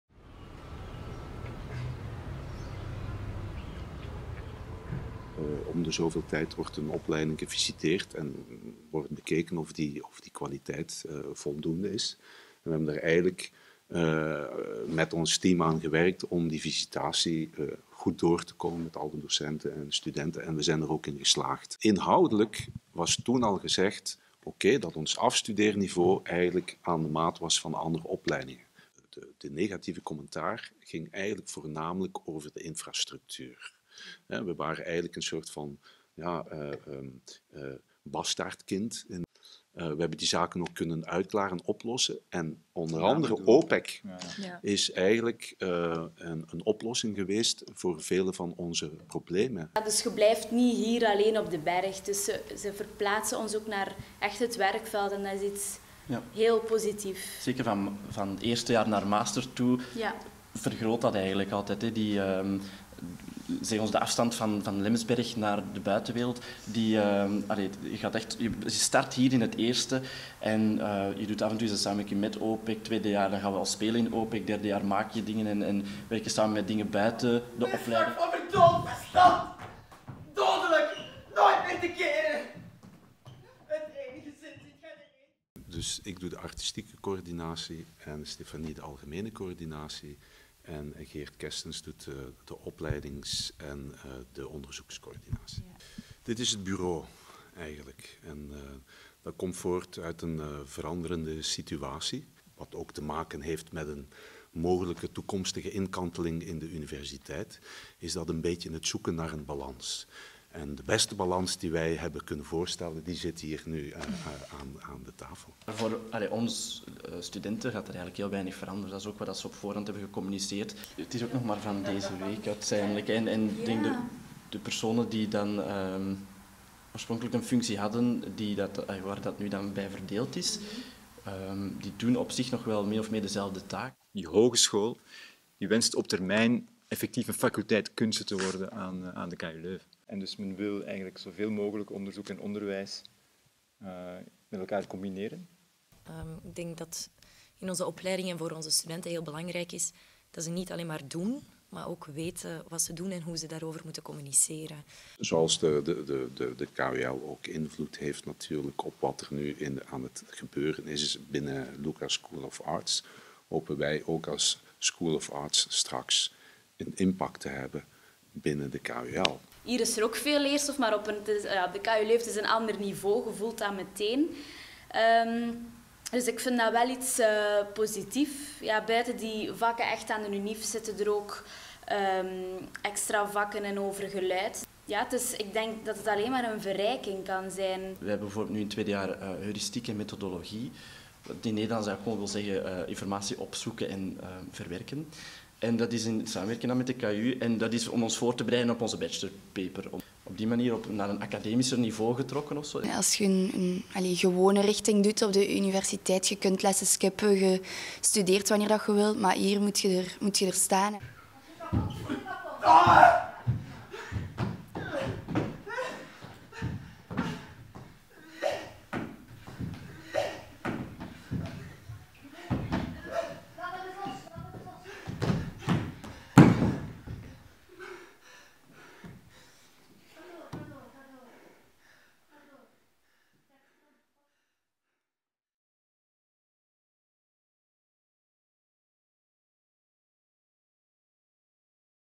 LUCA School of Arts Drama
Op een steenworp van het Gasthuisberg is er vanalles aan het veranderen.
-De dramaopleiding van LUCA School of Arts heeft er vorige week een positief tweede visitatierapport gekregen.
- Opleidingshoofd Kris Cuppens wordt vervangen door een triumviraat.
- Tegelijk worden allerlei maatregelen getroffen om de opleiding op universitair nivea te krijgen.